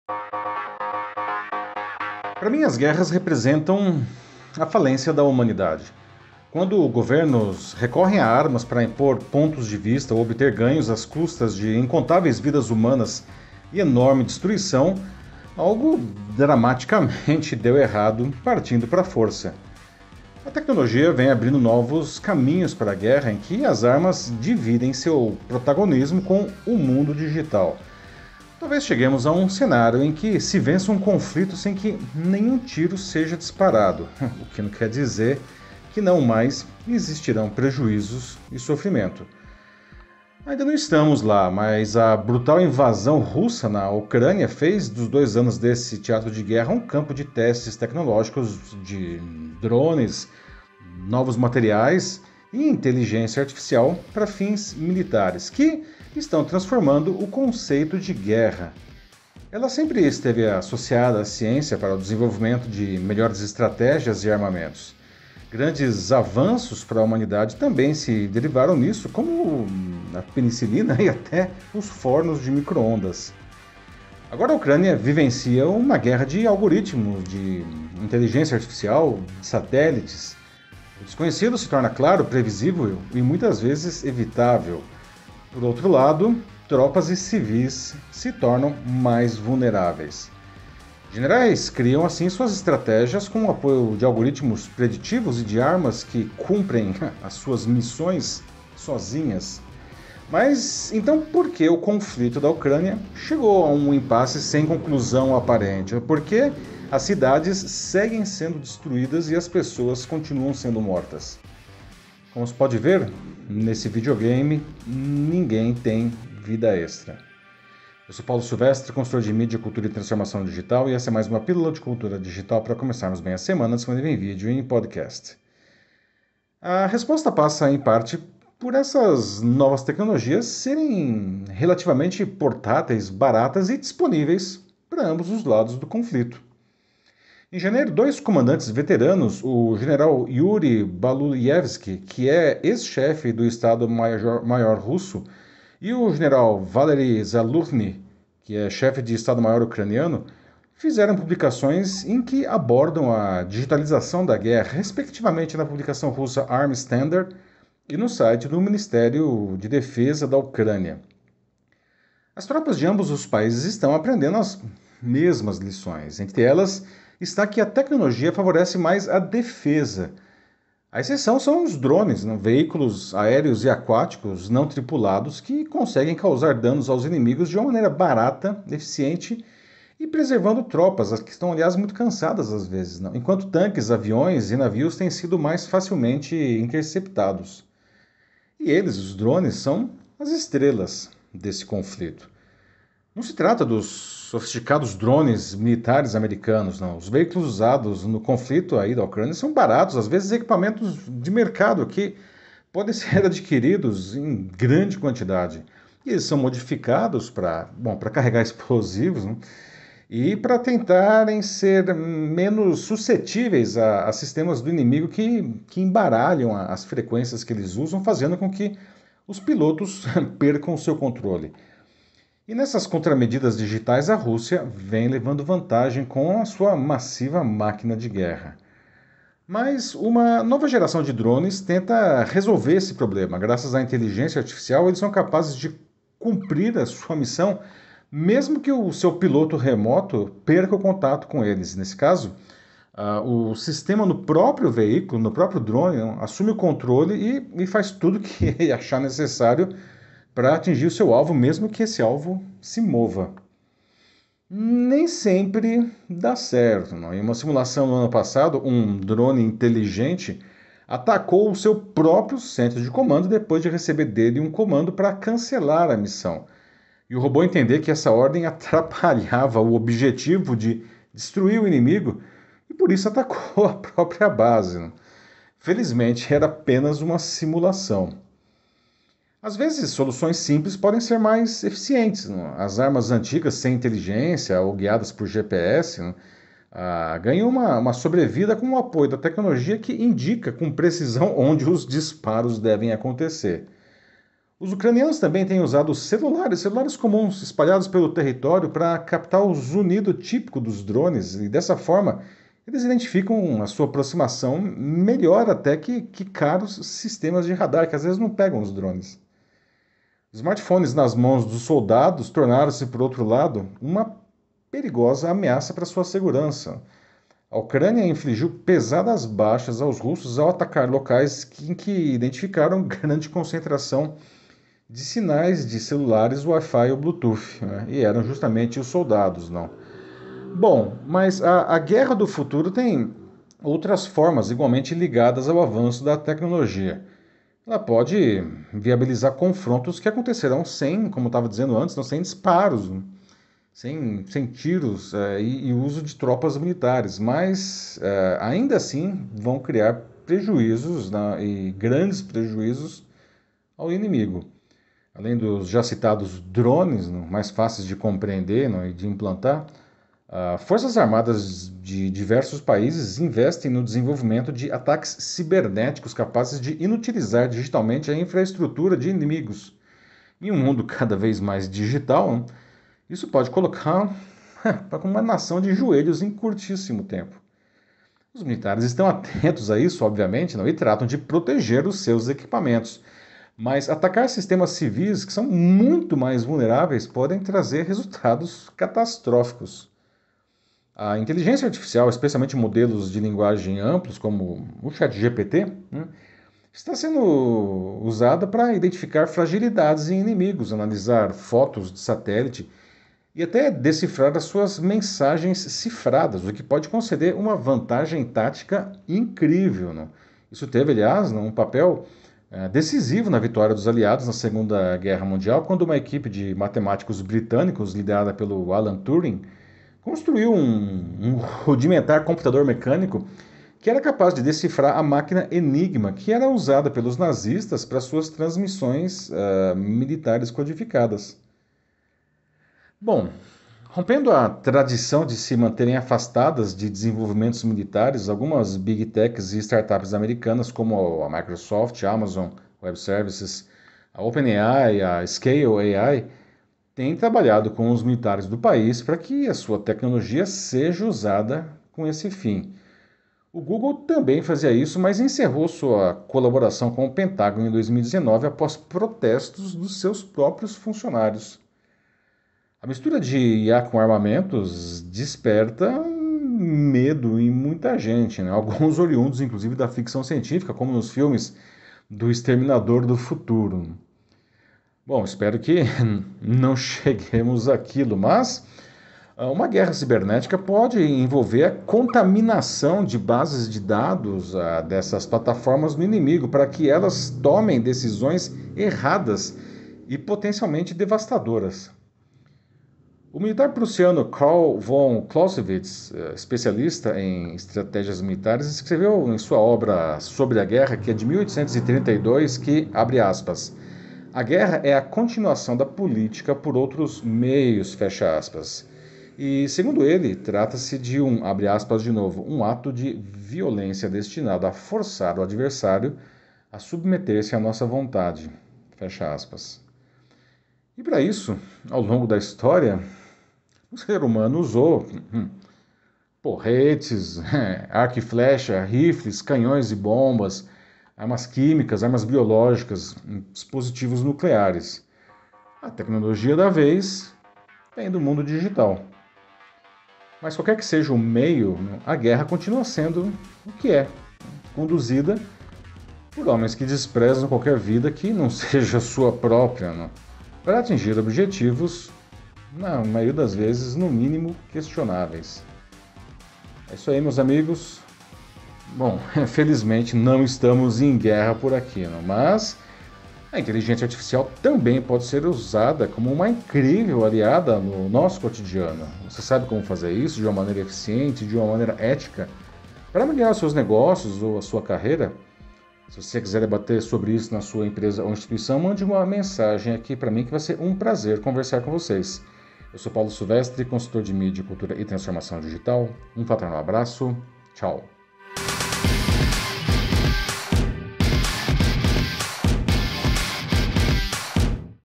As guerras do futuro (ou do presente?)
Pílula de cultura digital para começarmos bem a semana 😊
Como a inteligência artificial pode alterar o próprio conceito das guerras?

Para mim, elas representam a falência da humanidade. Quando governos recorrem a armas para impor pontos de vista ou obter ganhos às custas de incontáveis vidas humanas e enorme destruição, algo dramaticamente deu errado, partindo para a força.

A tecnologia vem abrindo novos caminhos para a guerra, em que as armas dividem seu protagonismo com o mundo digital.

Talvez cheguemos a um cenário em que se vença um conflito sem que nenhum tiro seja disparado, o que não quer dizer que não mais existirão prejuízos e sofrimento. Ainda não estamos lá, mas a brutal invasão russa na Ucrânia fez dos dois anos desse teatro de guerra um campo de testes tecnológicos de drones, novos materiais e inteligência artificial para fins militares, que estão transformando o conceito da guerra.

Ela sempre esteve associada à ciência, para o desenvolvimento de melhores estratégias e armamentos. Grandes avanços para a humanidade também se derivaram disso, como a penicilina e até os fornos de micro-ondas.

Agora a Ucrânia vivencia uma guerra de algoritmos, inteligência artificial e satélites. O desconhecido se torna claro, previsível e muitas vezes evitável. Por outro lado, tropas e civis se tornam mais vulneráveis. Generais criam assim suas estratégias com o apoio de algoritmos preditivos e de armas que cumprem “suas missões” sozinhas.

Mas então por que o conflito da Ucrânia chegou a um impasse, sem conclusão aparente? Por que as cidades seguem sendo destruídas e as pessoas continuam sendo mortas? É sobre isso e sobre o papel da tecnologia digital que abordo nesse vídeo.

Como se pode ver, nesse videogame, ninguém tem vida extra.